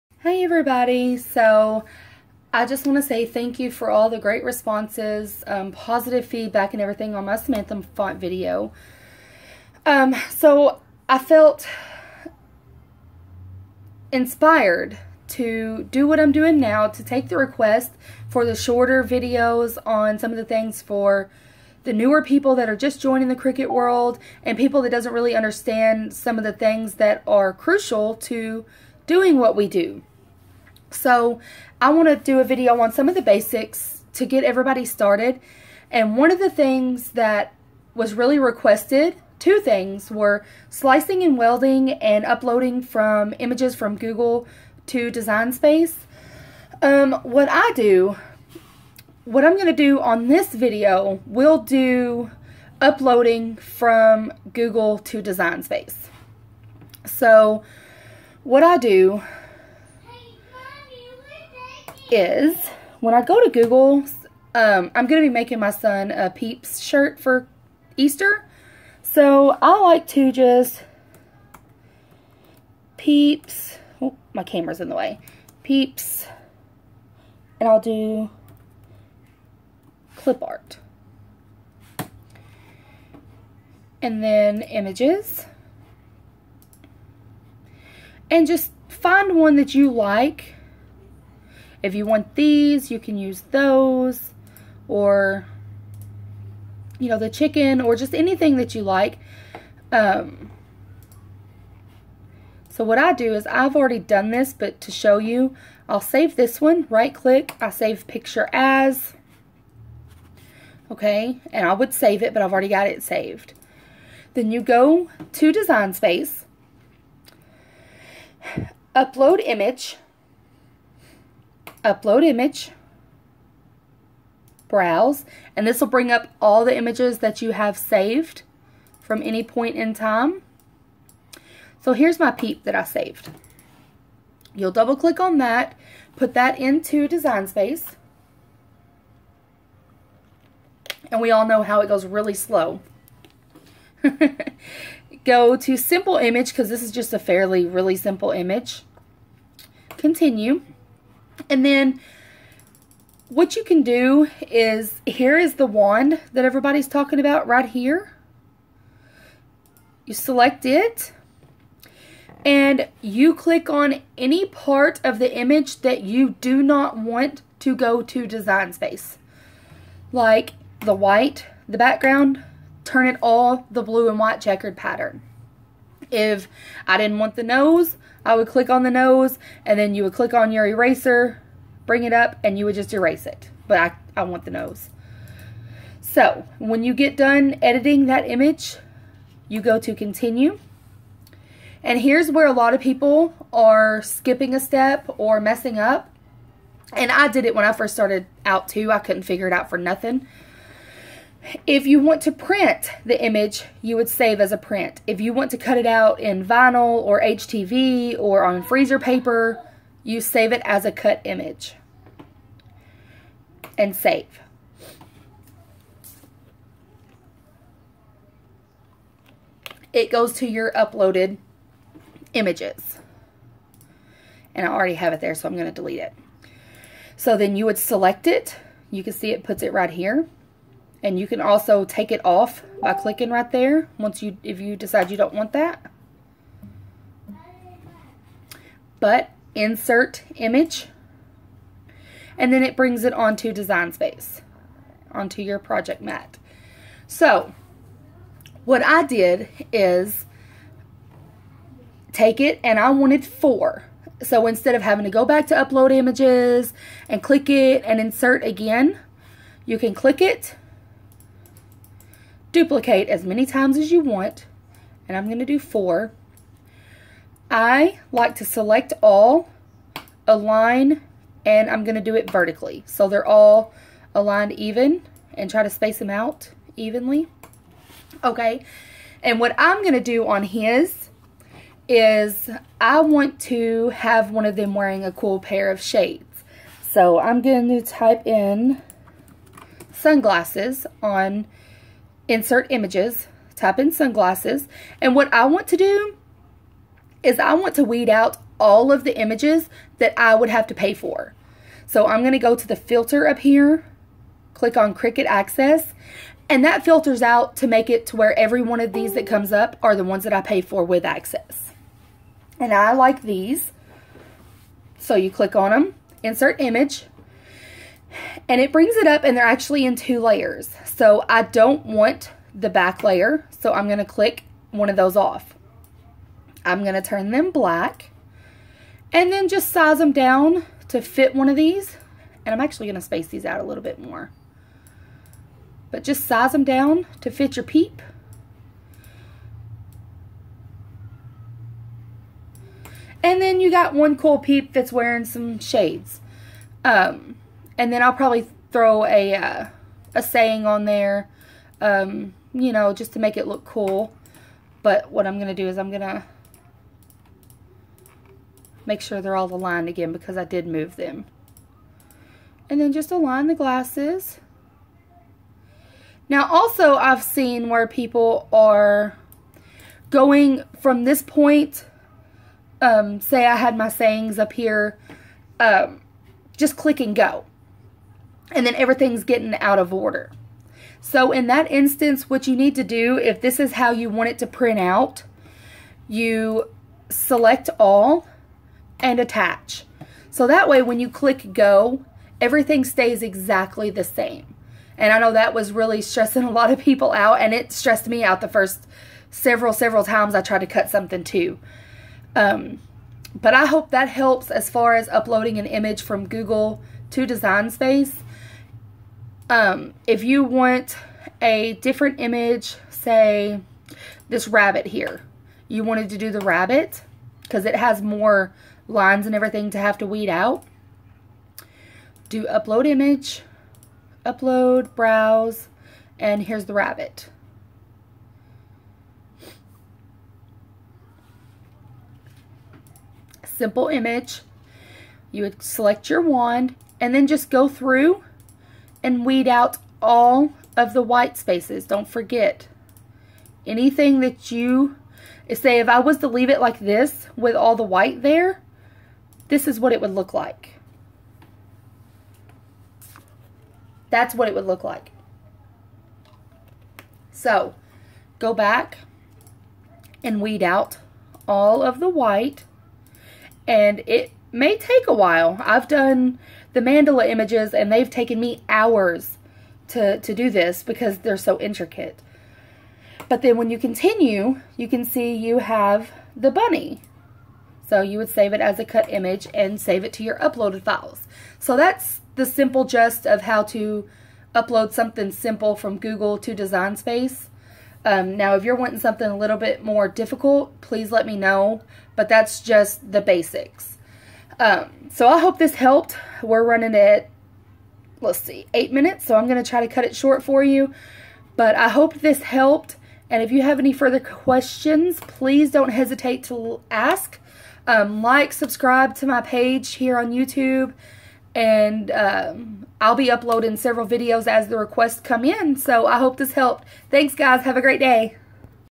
Uploading images from Google to design space
This will teach you how to upload from Google to design space with simple images